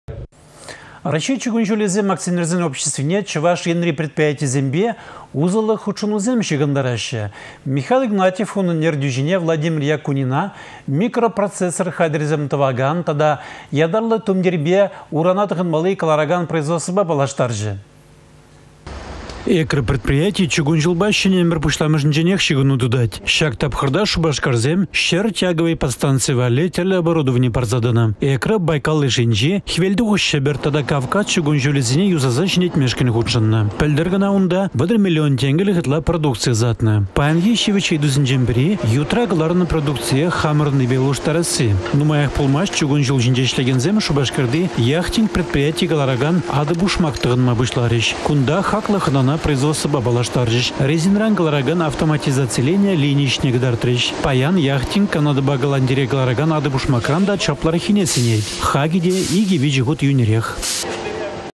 Игнатьевпа Якунин Экрăпа Яхтингра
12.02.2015 «Раççей чукун çулĕсем» акционерсен обществине Чăваш Енри предприятийĕсемпе усăллă хутшăнусем çыхăнтараççĕ. Михаил Игнатьев Владимир Якунина микропроцессор хатĕрĕсем тăвакан тата ятарлă тумтирпе урана тăхăнмалли кăларакан производствăпа та паллаштарчĕ.